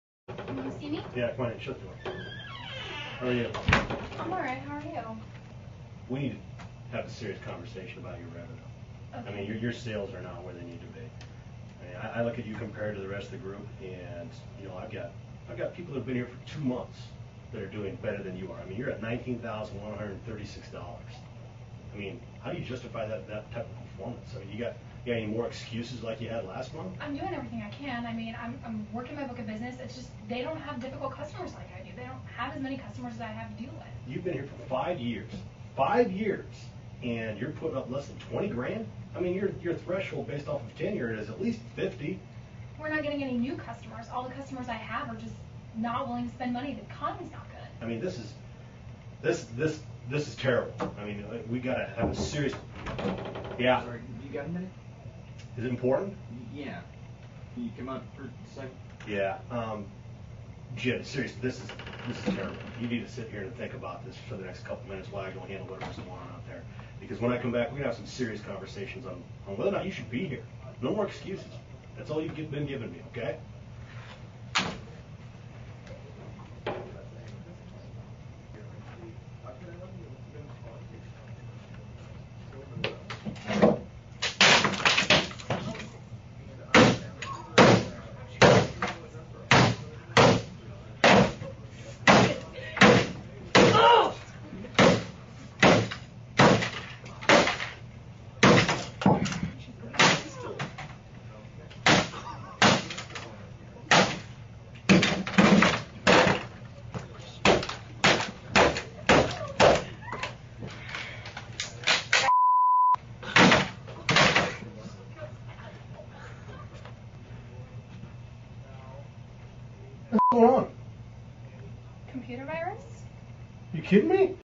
Woman Freaks Out On Her Boss
This chick goes berserk once she finds out she's about to be fired. Her boss is going to need a new laptop!